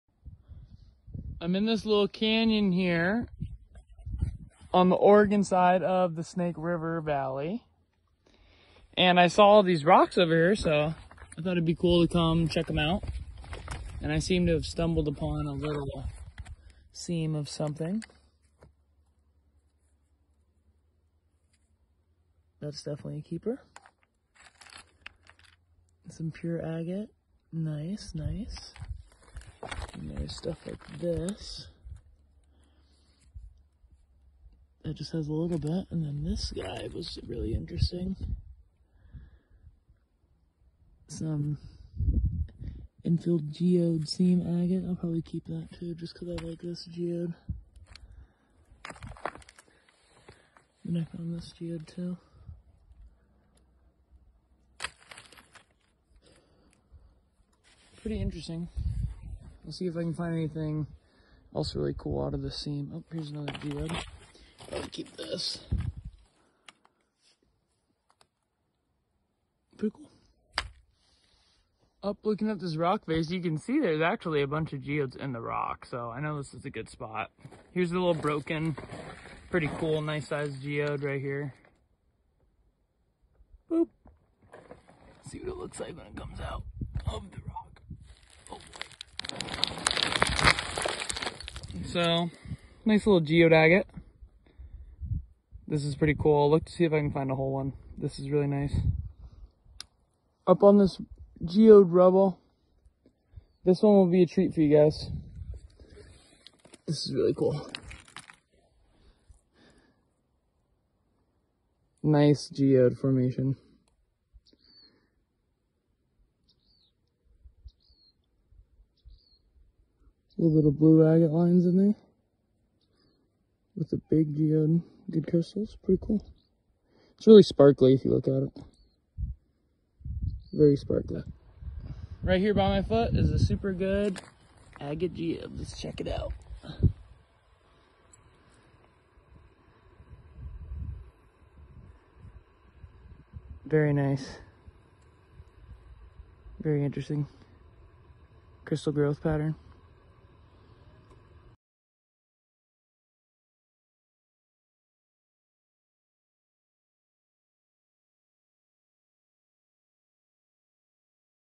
Geodes in Snake Valley, Oregon
Here I am around Snake Valley River in Oregon finding some cool geode chunks! oregon rockhounding nature geode rock @mineralsandmarathons